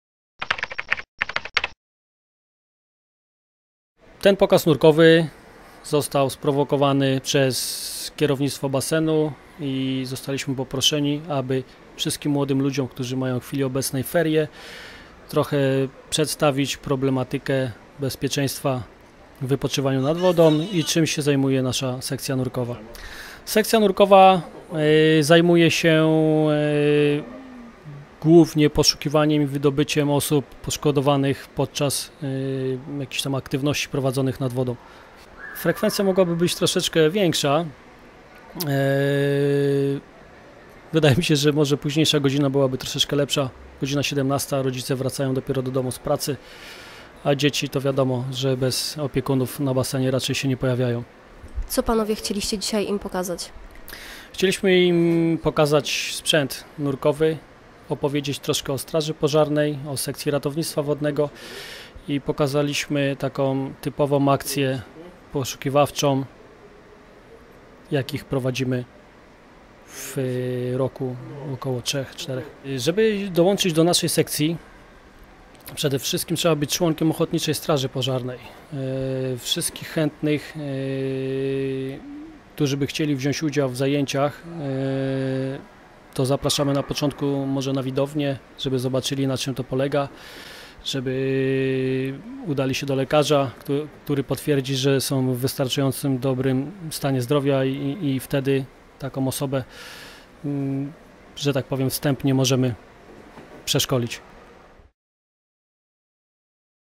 Podwodny pokaz strażaków ochotników z Sekcji Ratownictwa Wodnego OSP/ erawa.pl
Wyjątkowy pokaz mogli zobaczyć mieszkańcy miasta, którzy korzystali dziś z rawskiego basenu. Strażacy ochotnicy, reprezentujący Sekcję Ratownictwa Wodnego OSP Rawa Mazowiecka, prezentowali swoje techniczne i sprzętowe możliwości. Bardziej zainteresowani rawianie swoich sił mogli nawet spróbować z aparaturą tlenową na plecach.